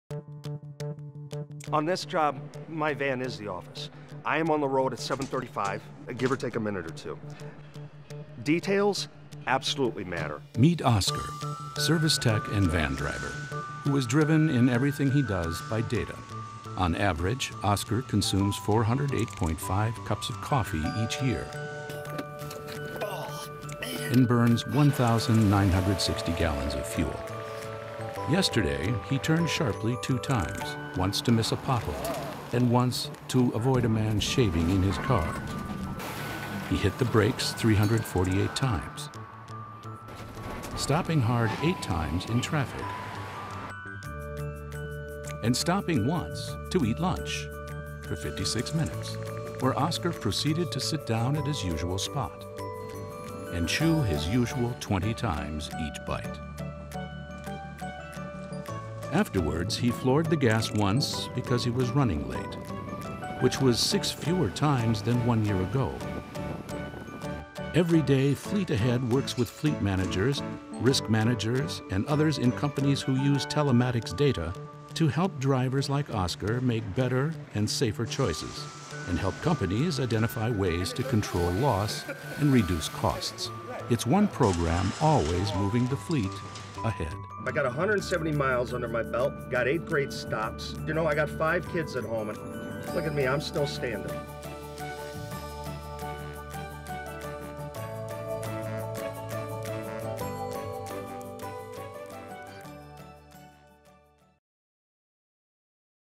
FleetAhead®: Where data analysis drives fleet management | The Hartford
Driving is inherently risky. FleetAhead from The Hartford can help manage your fleet's risk by combining the technology of telematics with The Hartford's Risk Engineering Services.